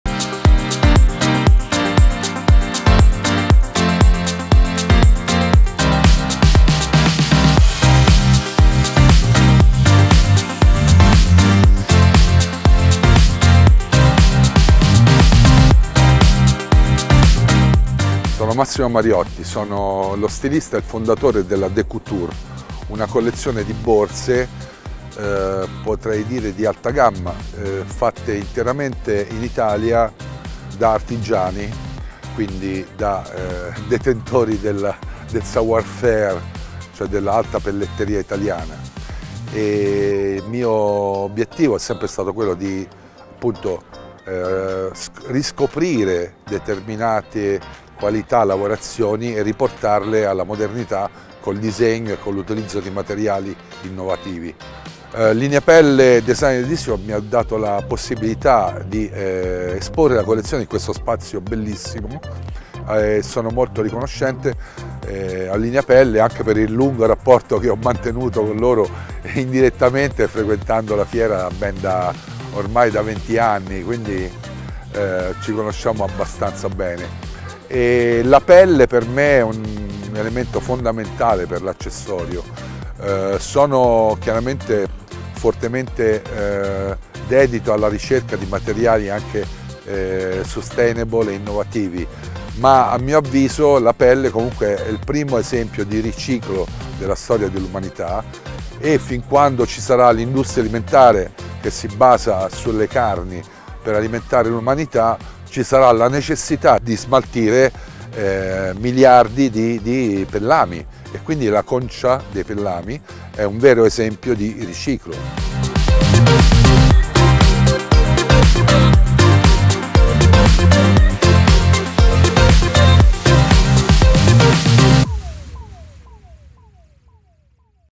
LINEAPELLE DESIGNERS EDITION_DE COUTURE
LINEAPELLE DESIGNERS EDITION, an event created to support and promote leather and its most original and creative applications.

25 FEBRUARY 2024
DE COUTURE
“Savoir-faire”

SPAZIO LINEAPELLE
Milano, Piazza Giuseppe Tomasi di Lampedusa